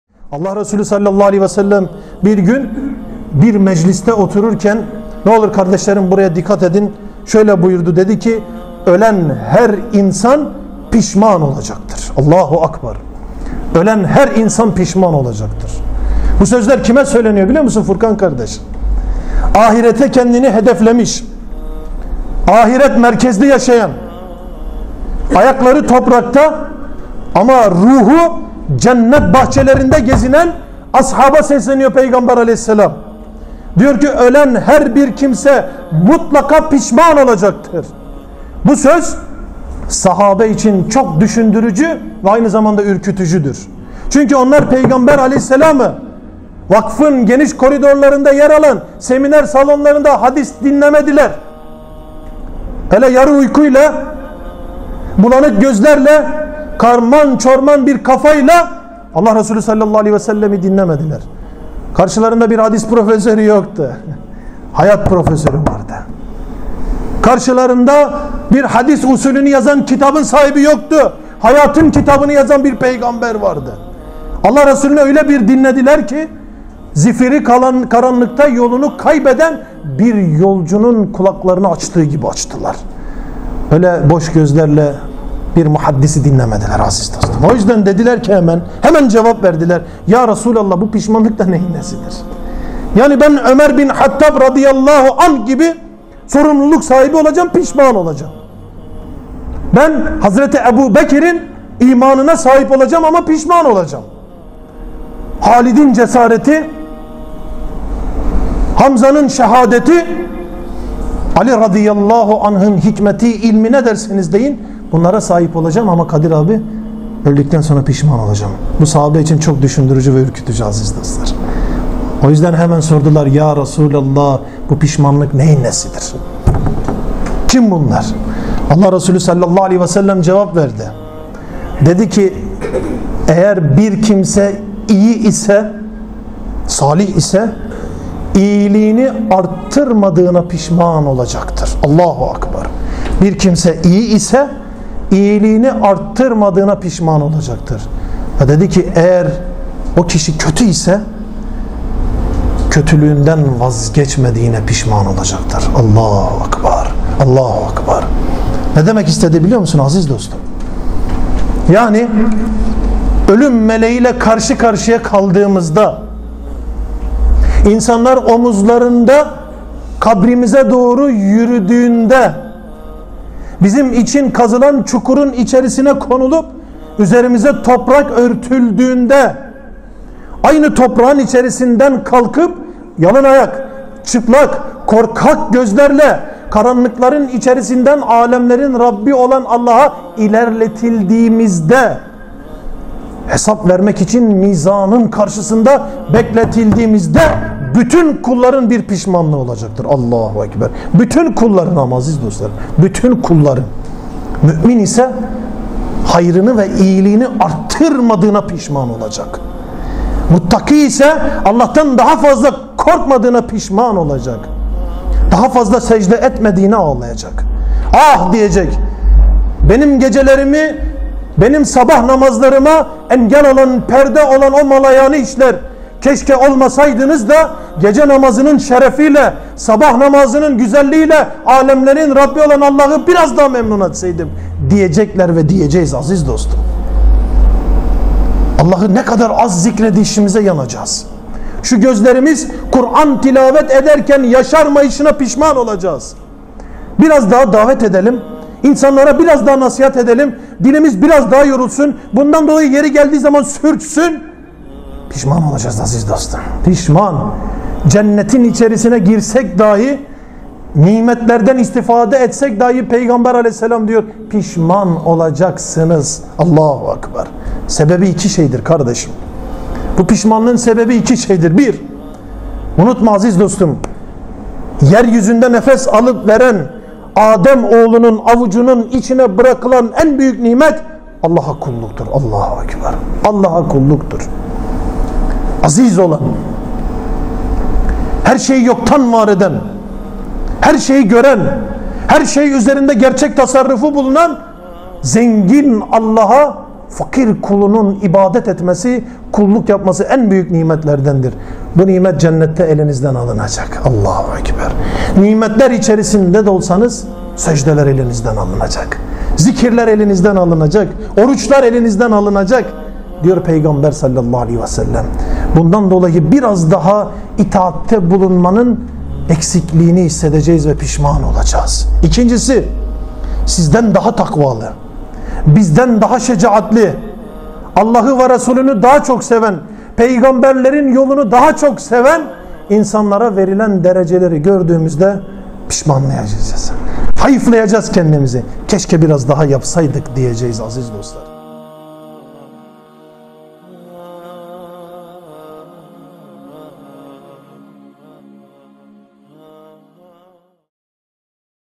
Neden Ölen Her İnsan Pişman Olacak?
Bu kesit Allah Kimlere Rahmet Eder başlıklı linkli sohbetimizden alınmıştır.